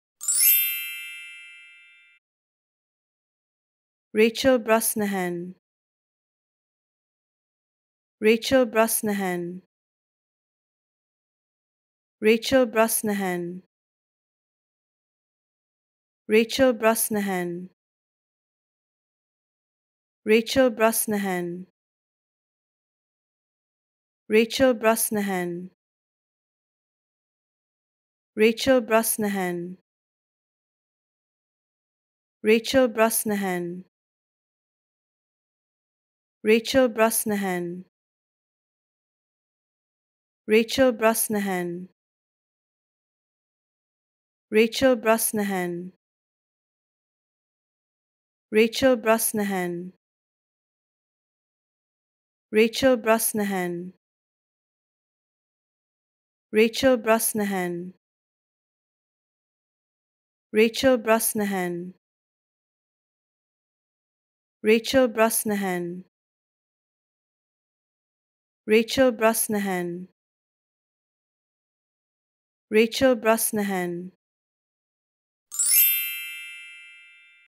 How to Pronounce Rachel Brosnahan
Who is Rachel Brosnahan? Learn how to pronounce the word 'Rachel Brosnahan' with Unstammer's free pronunciation tutorials.